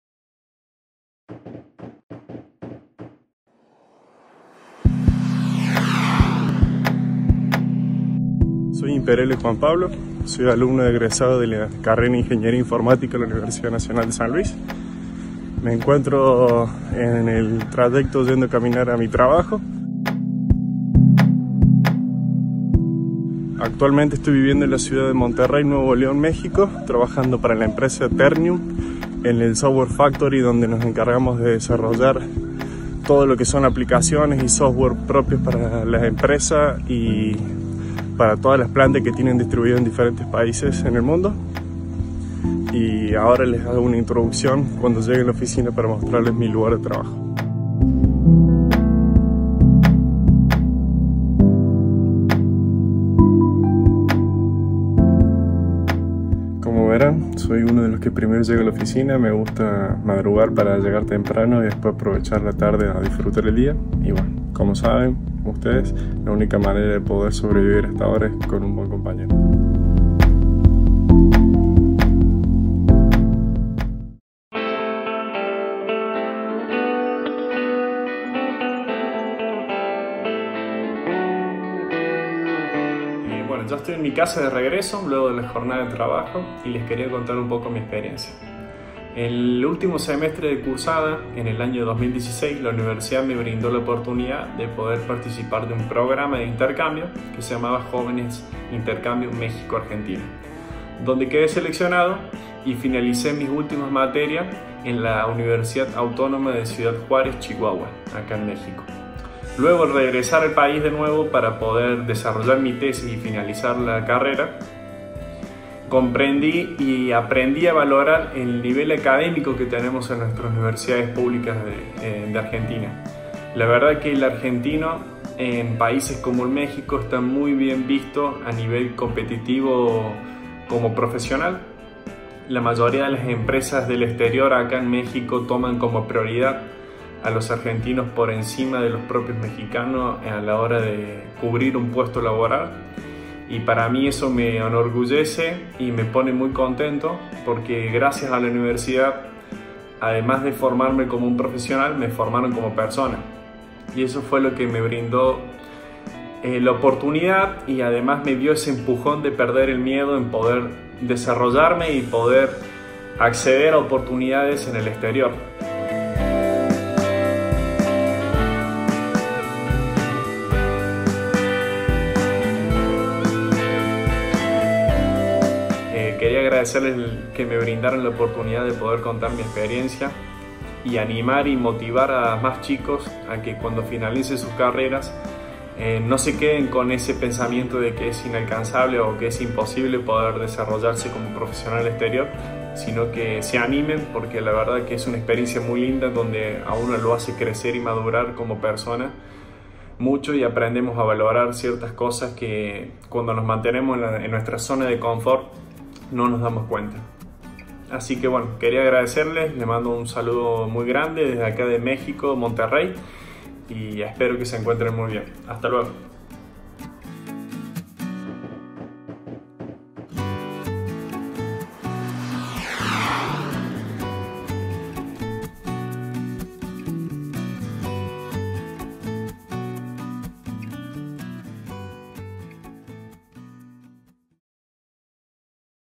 Viaje de Egresados - Juan Imperiale
Viaje de Egresados es una serie web que muestra un día en la vida de diferentes egresados y egresadas de la Universidad Nacional de San Luis. Los/as protagonistas se encuentran trabajando o completando su formación fuera de la provincia y muestran en primera persona, su rutina diaria y su experiencia en el exterior.

En este primer capítulo conocemos a Juan Imperiale, egresado de la carrera de Ingeniería en Informática de la Universidad Nacional de San Luis. Nos mostrará su día a día en la ciudad de Monterrey, México.